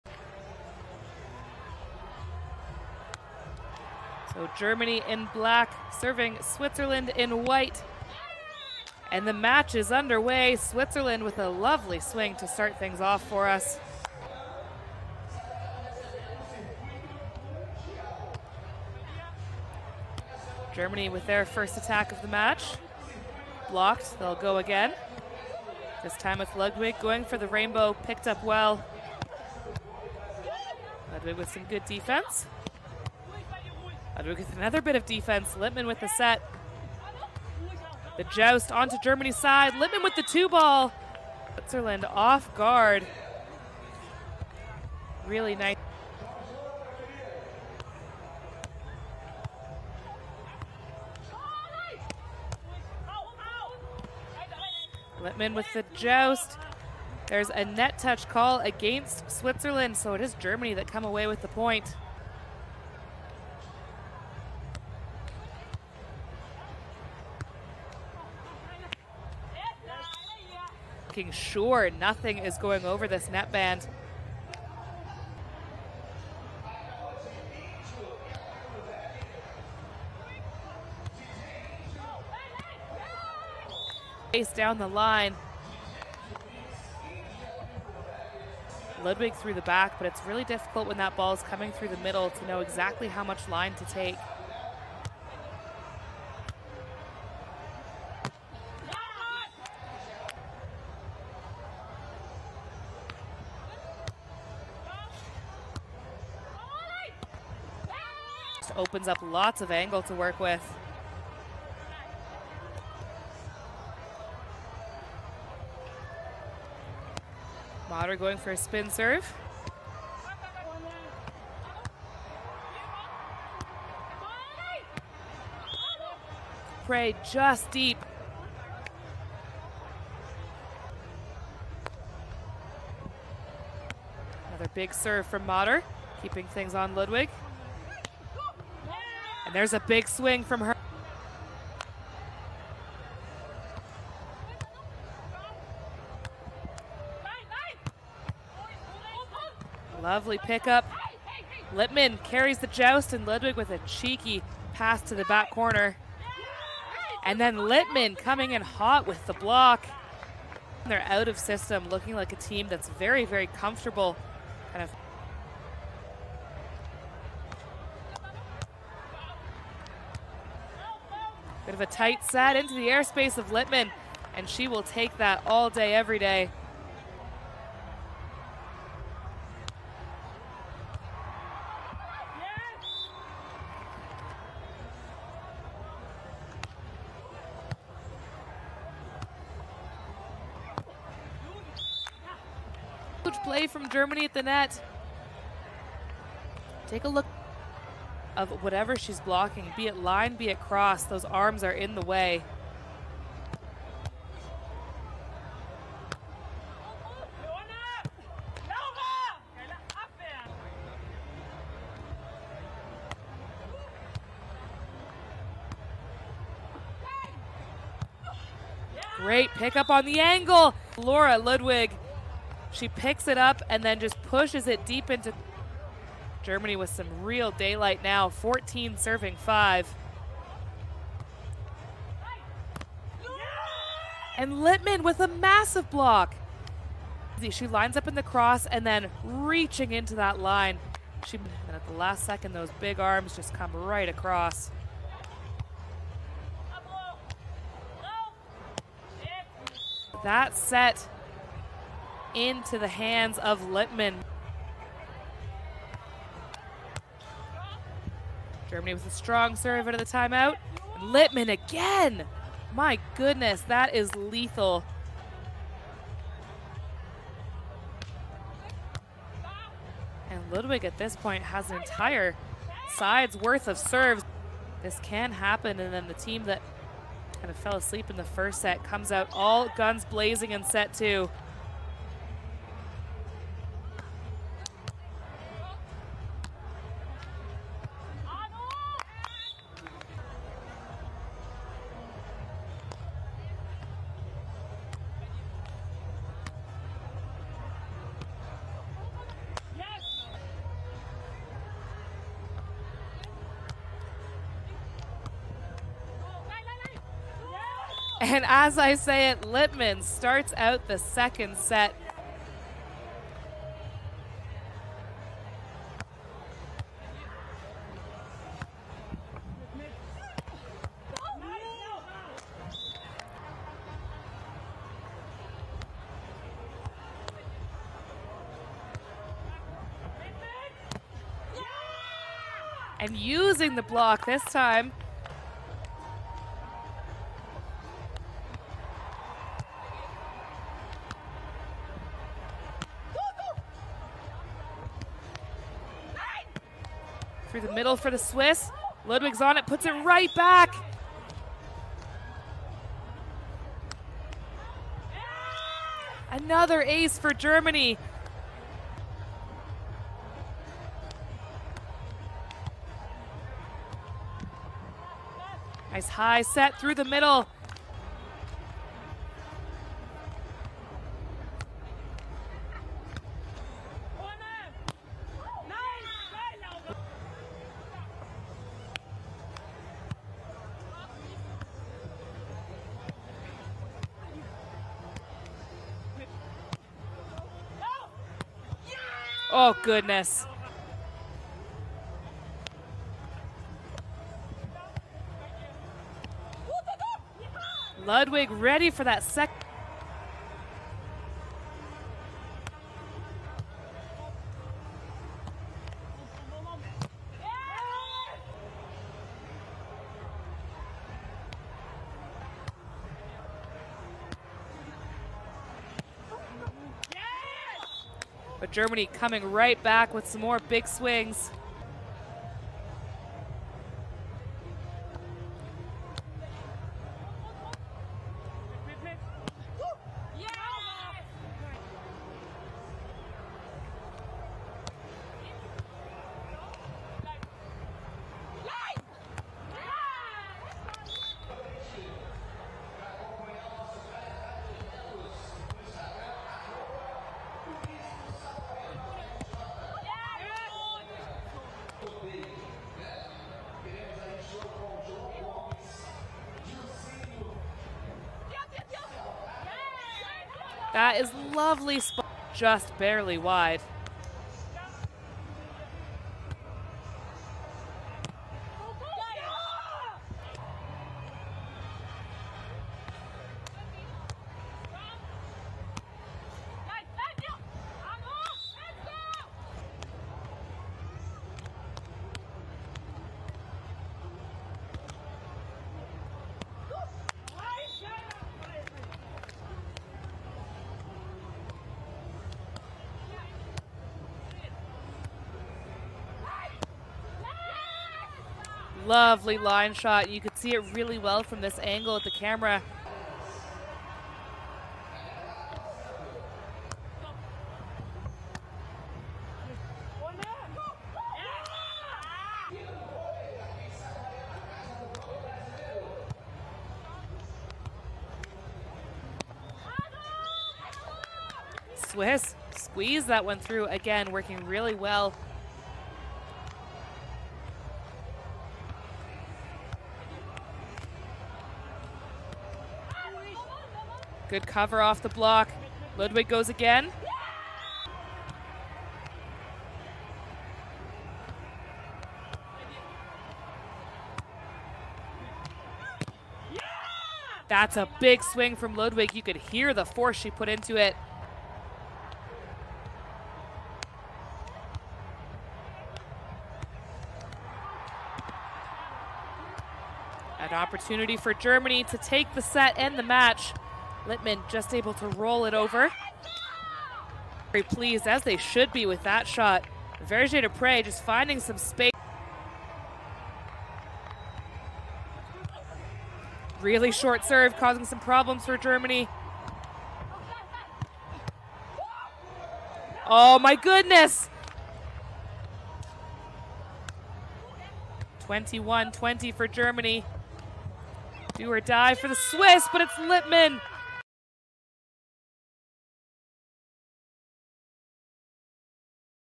Vergé-Dépré A./Mäder vs. Ludwig/Lippmann - Quarter Finals Highlights Saquarema 2024
Enjoy watching the Women's Quarter Finals highlights of the Challenge tournament in Saquarema (Brazil), part of the 2023 Beach Pro Tour!

LOVE VOLLEYBALL?